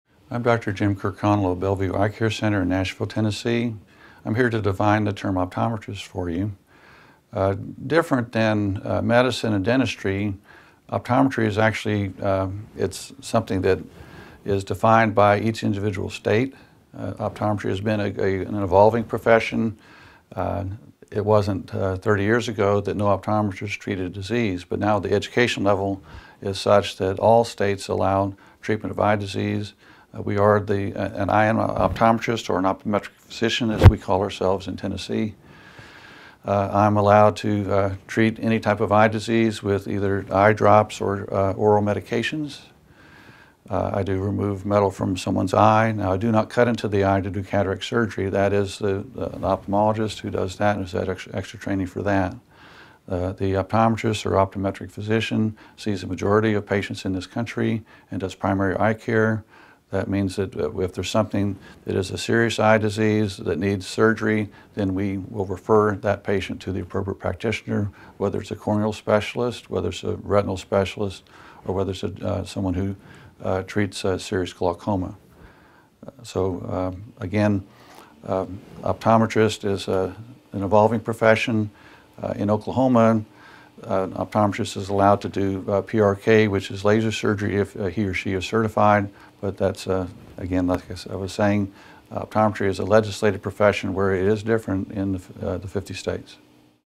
Eye Health & Optometry : What Is an Optometrist?
An optometrist is a physician who treats vision impairments through contact lenses or eyeglasses, but one is also trained to diagnose eye diseases and extract foreign objects from the eye. Understand an optometrist's job description with information from a practicing optometrist in this free video on eye health.

Expert: James W. Kirkconnell
Bio: Dr. James W. Kirkconnell graduated from the University of Houston College of Optometry in 1984.